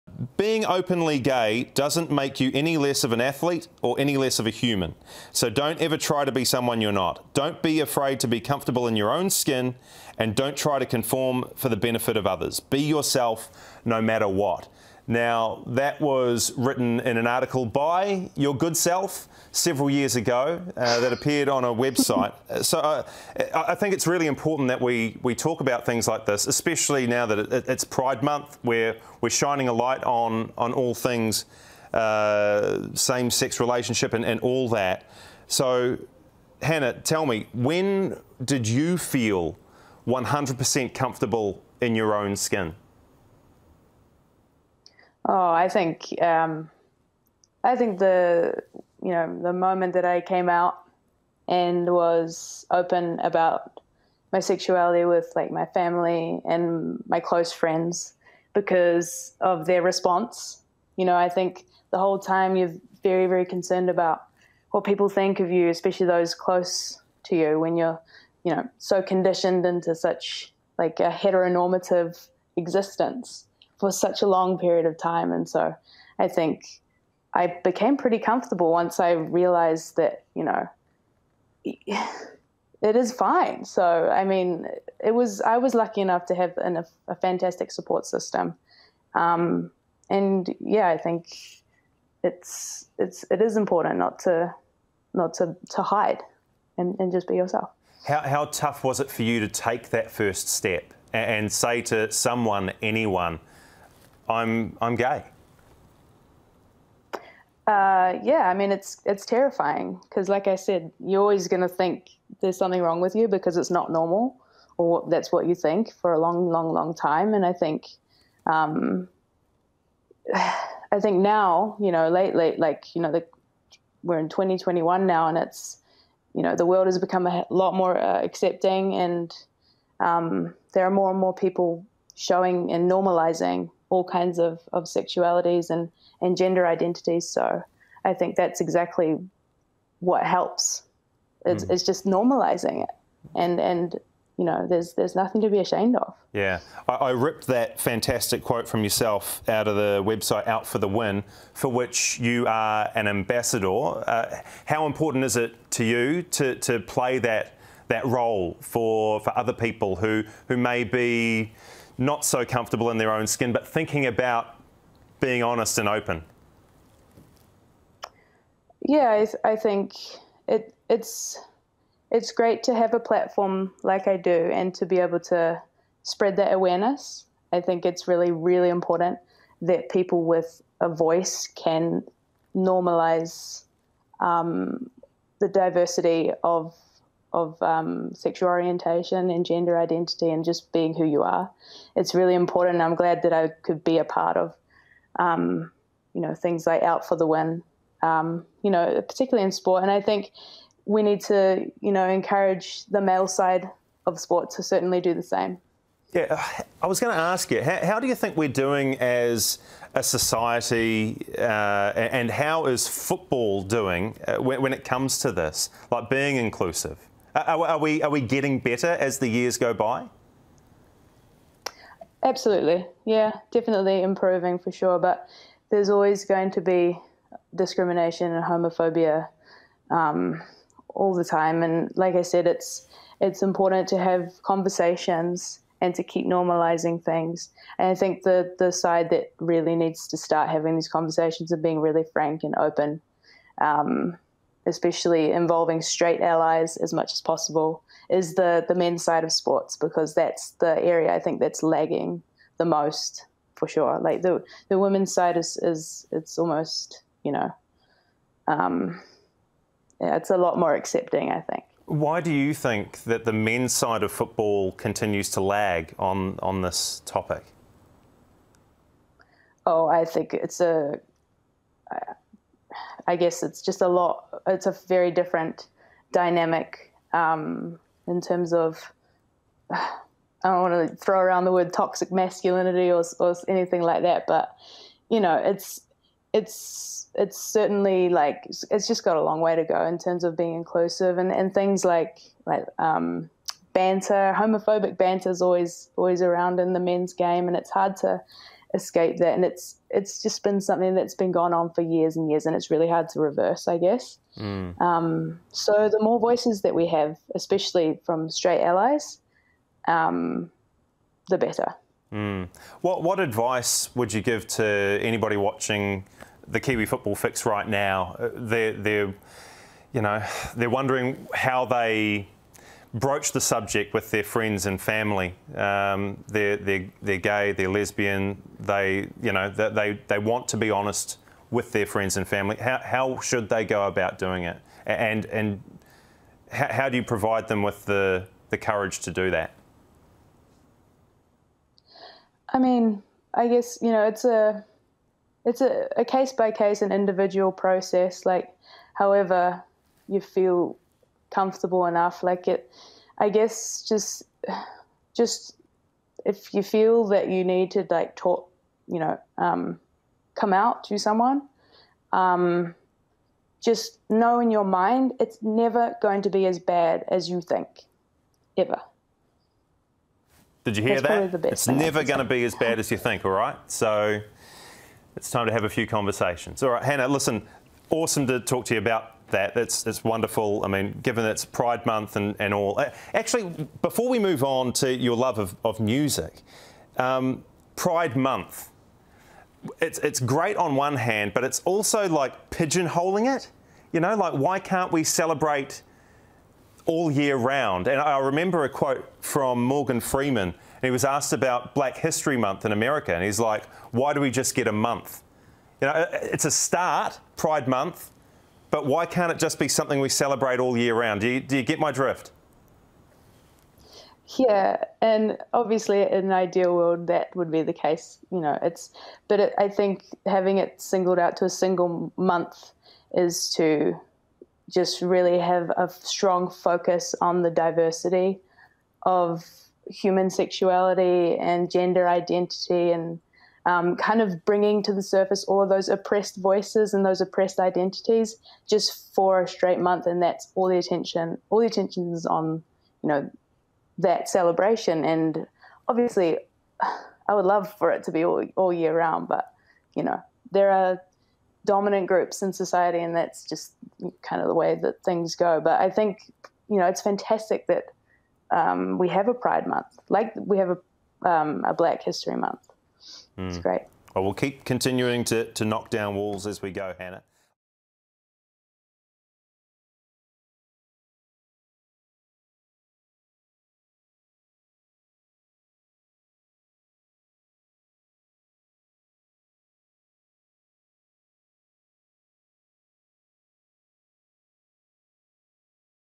Hannah Wilkinson on inclusivity
Football Ferns striker Hannah Wilkinson joins @skysportnz Kiwi Football Fix host Goran Paladin to talk about being yourself, coming out and Pride Month.